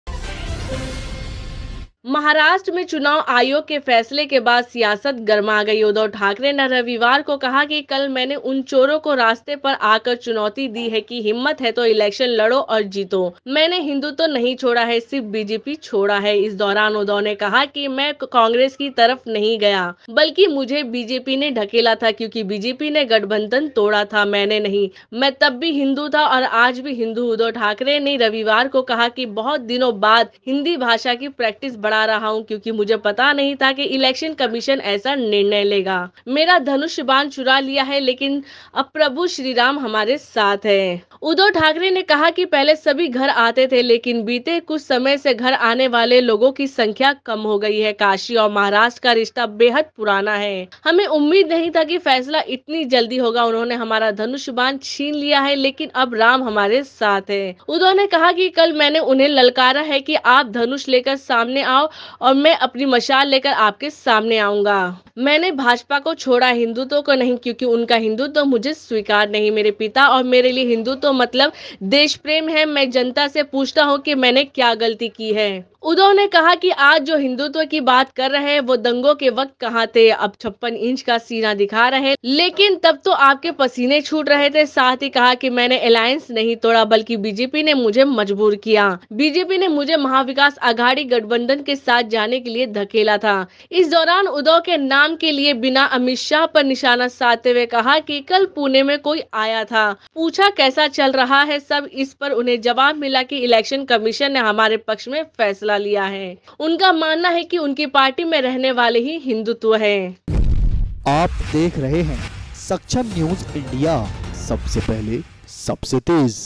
मैं कांग्रेस की तरफ नहीं गया, BJP ने धकेला था', शिवसेना पर EC के फैसले के बाद बोले उद्धव | SNI
मैं कांग्रेस की तरफ नहीं गया, BJP ने धकेला था', शिवसेना पर EC के फैसले के बाद बोले उद्धव